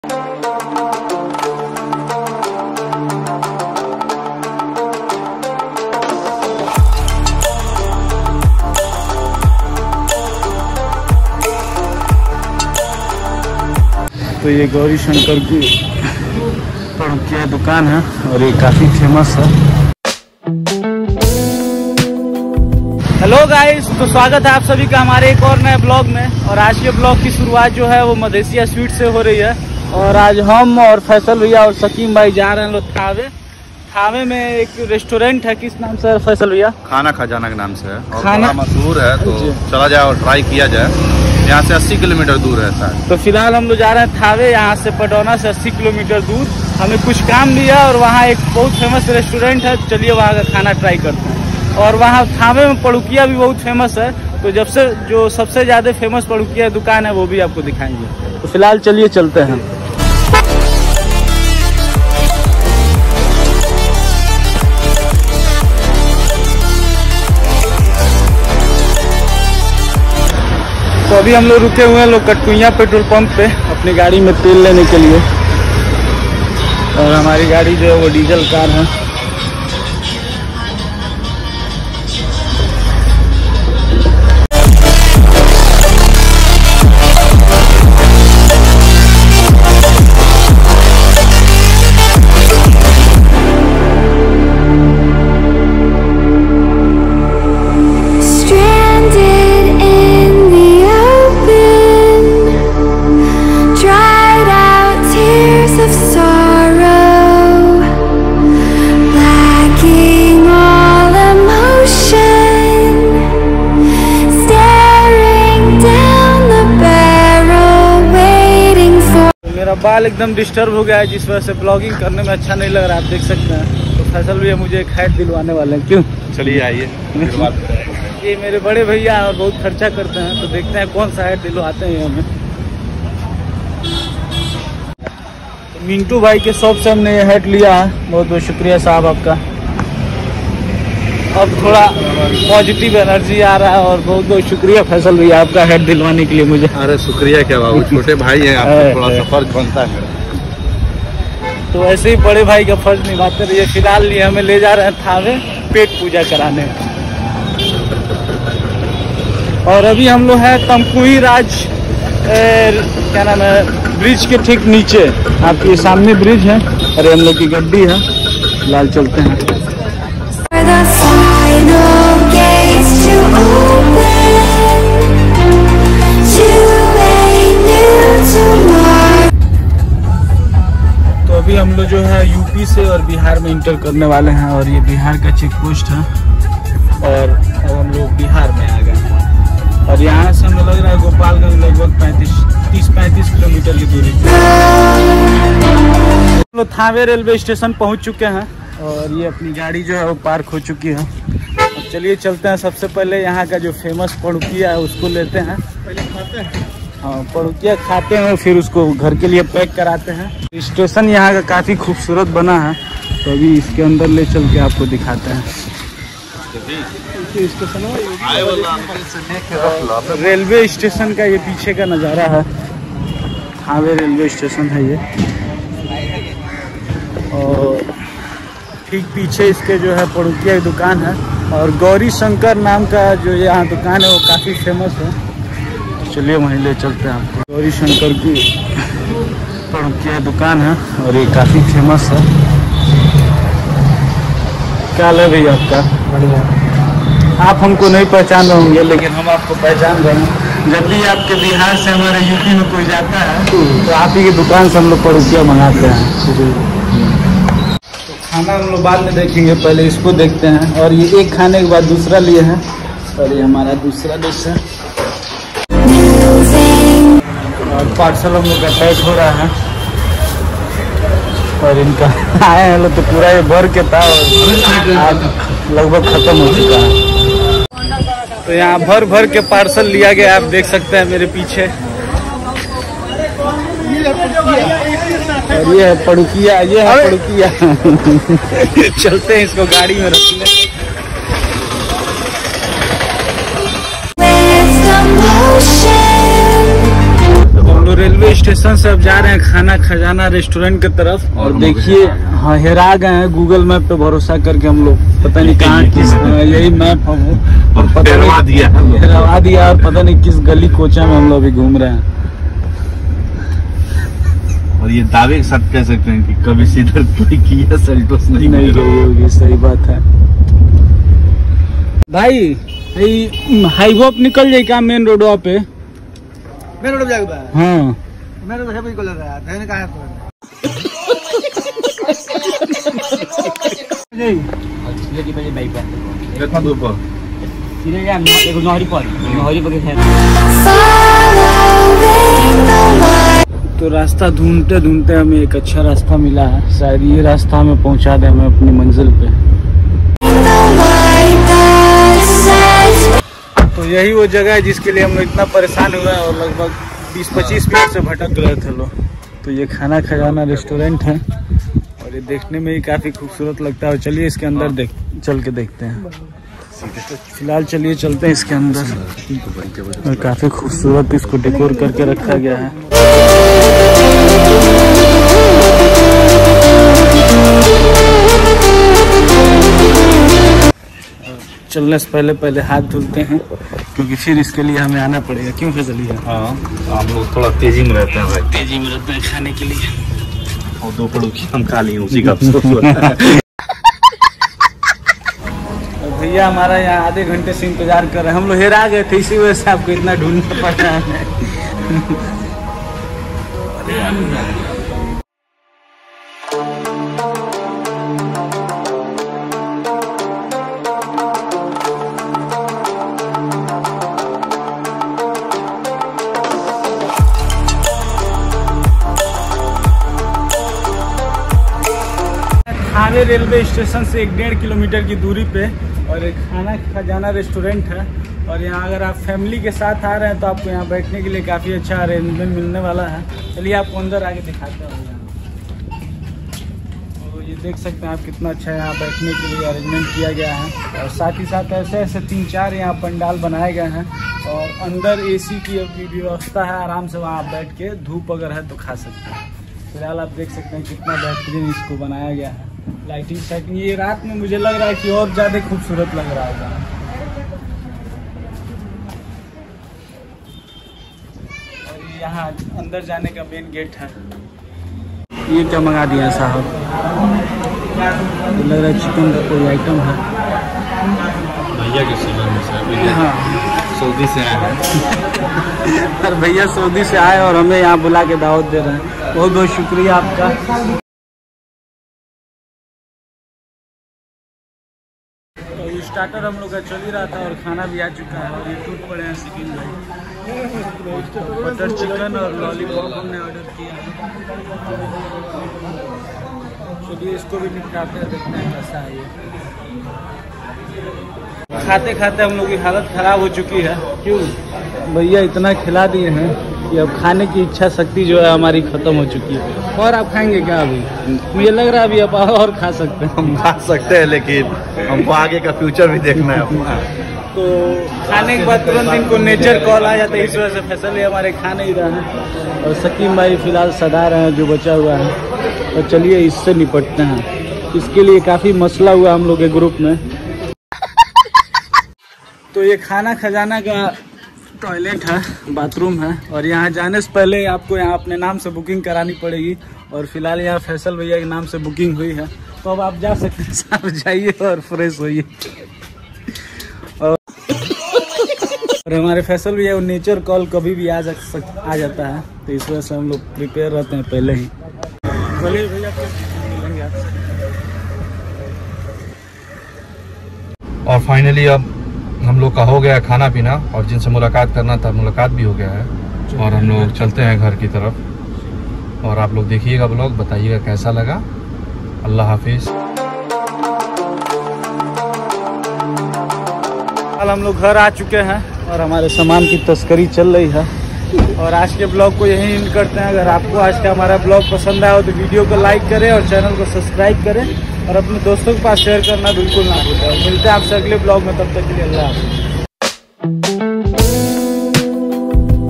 Padrauna To Thawe Bihar😎|Khana Khajana| Gauri Sankar Padukiya| Arif Vlogs| Kia Seltosh| Family Vlogs
Padrauna To Bihar Thawe
Khana Khajan Restorent Thawe. Near Gopal Gajn 🛣️
Thawe Railway Station Back Side view😎
Famous Padukia Shop In Thawe Bihar
Gauri Sankar Swets Padukiya Wale…
Patel Belts & Helmet Centar Tamkuhiraj

Best Swets In Bihar
Bihar ka Famus Mitahi

Exploring Khana Khajana Restoren Thawe Bihar

Aap Ko Hamra Aaj Ka Vlog Kaisa Laga😁

Aap Ko Mera Vlog Kaisa Lagta Hai... 🤔